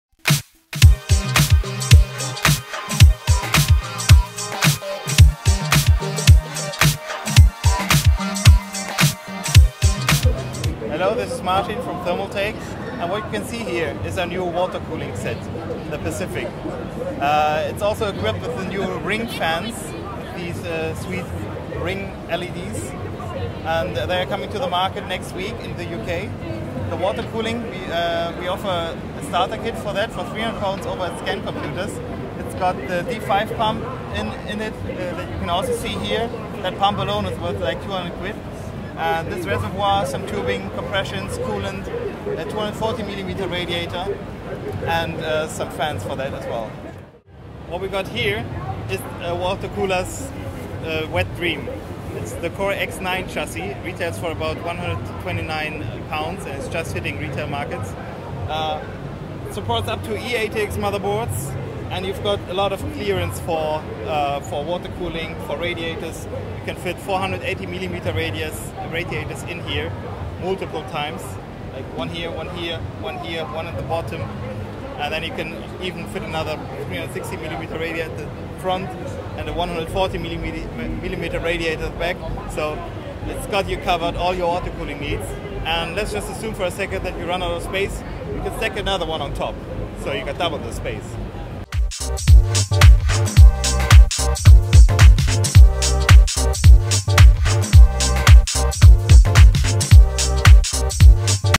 Thermaltake Pacific Water Cooling Kit and Core X9 Chassis Overview
I'm back one last time with our final bit of coverage on Insomnia I54. We interviewed Martin from Thermaltake about the new Pacific Water Cooling Kit and the new Thermaltake Core X9 Chassis.

I hope you enjoyed the video but the buzz doesn't stop here!